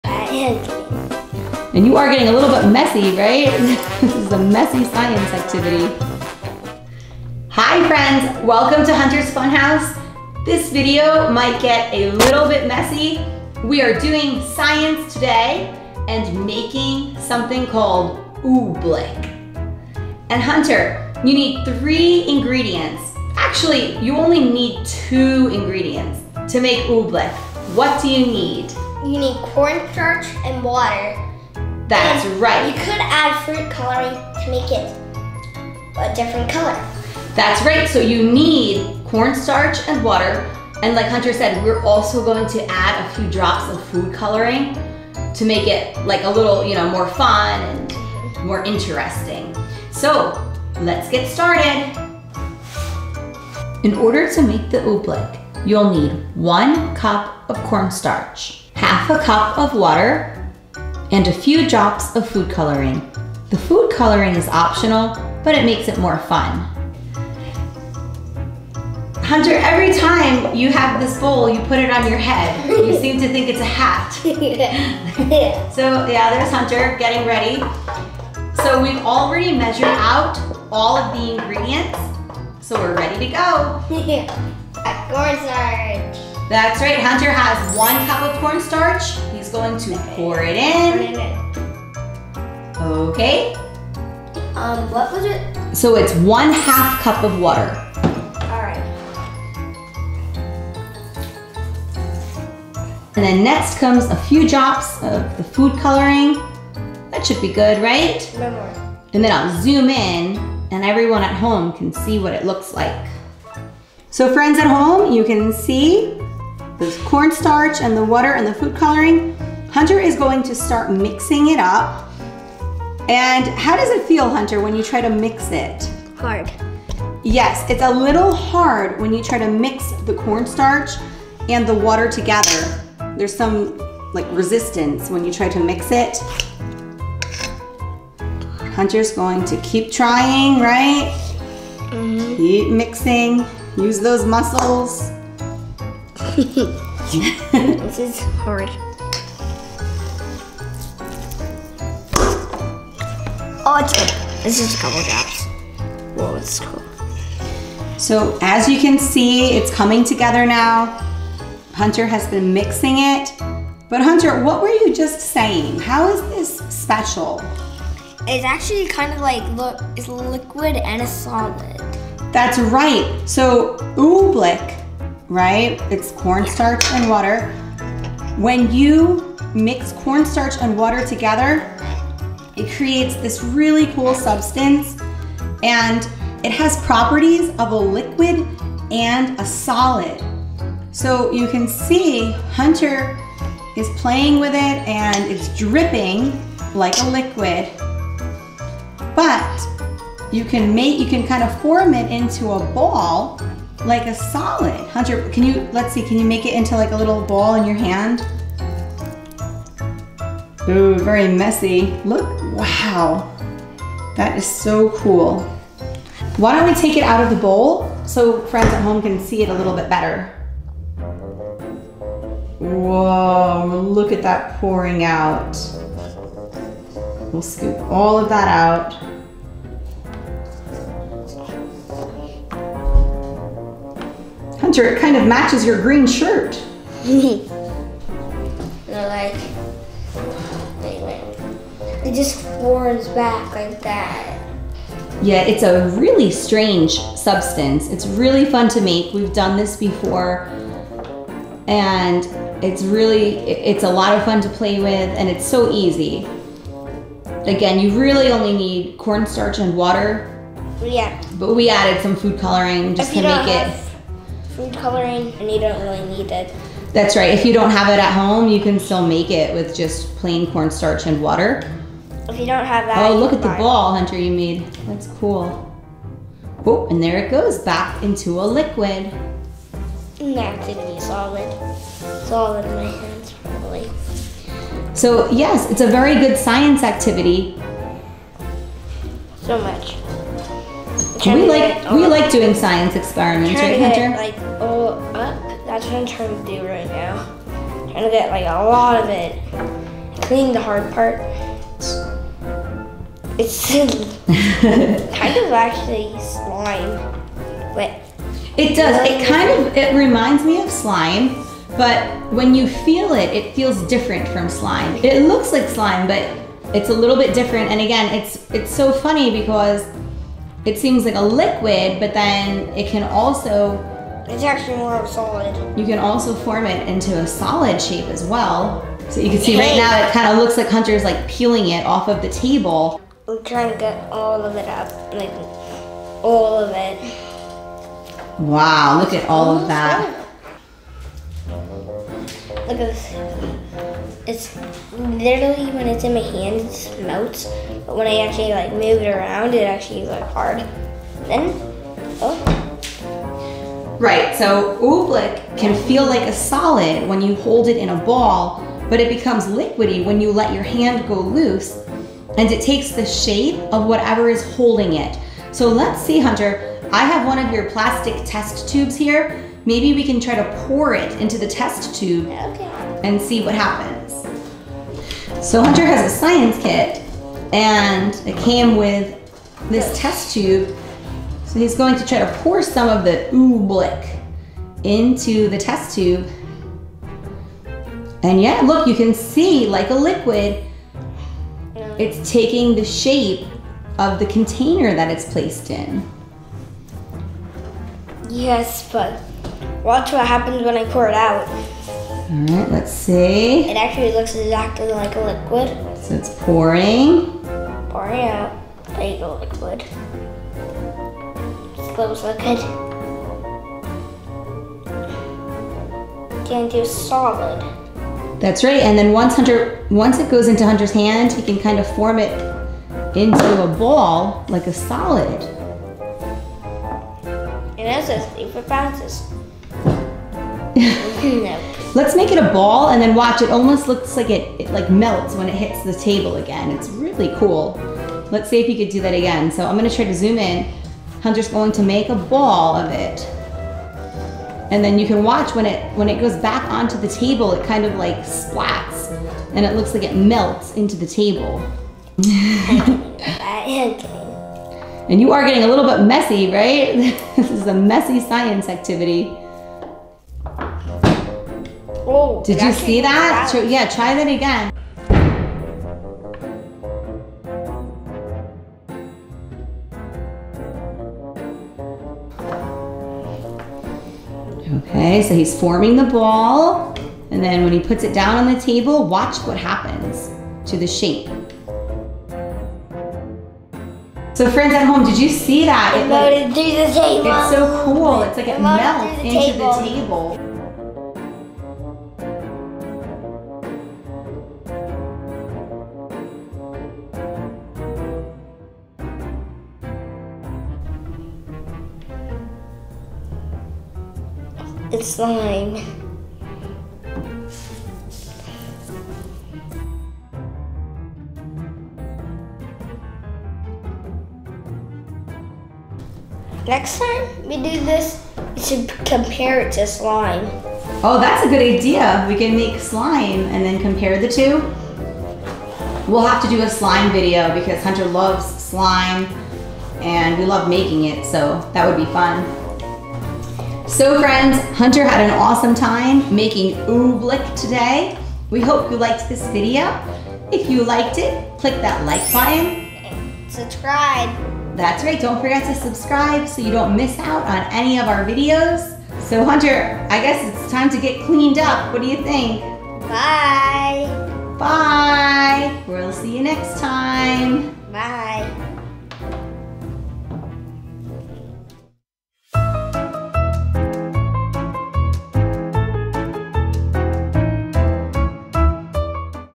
Making Oobleck! Sloppy Fun Science For Kids!
In this video, Hunter makes a substance called Oobleck!  Making Oobleck is a great Science / STEM activity for children!  You simply mix together cornstarch and water - food coloring is optional, but FUN!  Oobleck has properties of both a liquid and a solid - it drips like a liquid but can also be formed into a ball.  It's a great way to teach children about different states of matter.  Don't miss out on this fun and educational STEM video!  Warning - you may get messy!!